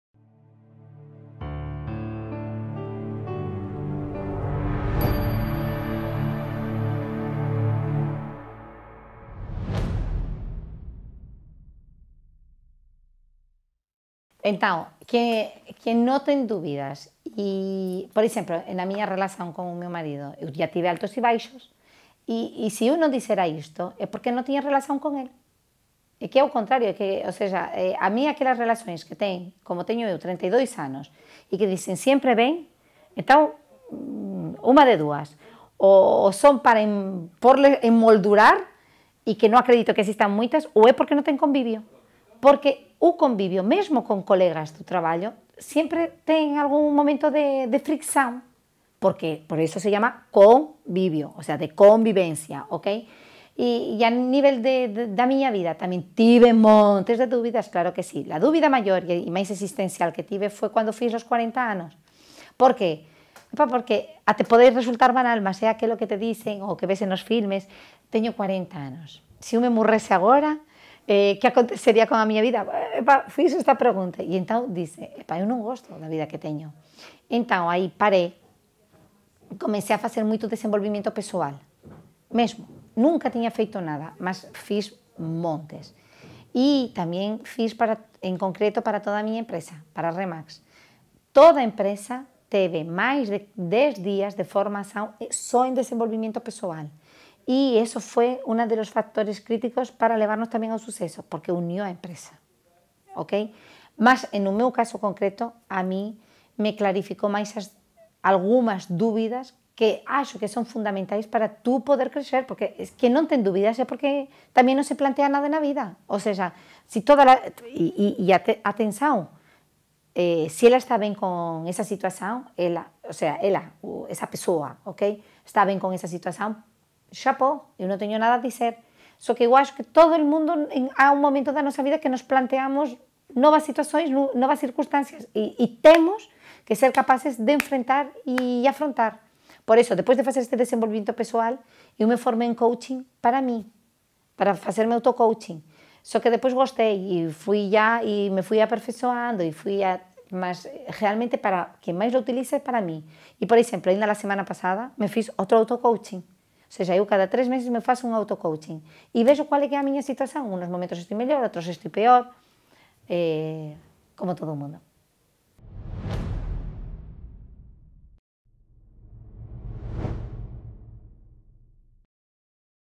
Beatriz Rubio: Houve algum momento de dúvida sobre se estava no caminho certo?
Entrevista completa com Beatriz Rubio, CEO da Remax Portugal e Motiva-te, no Portal da Liderança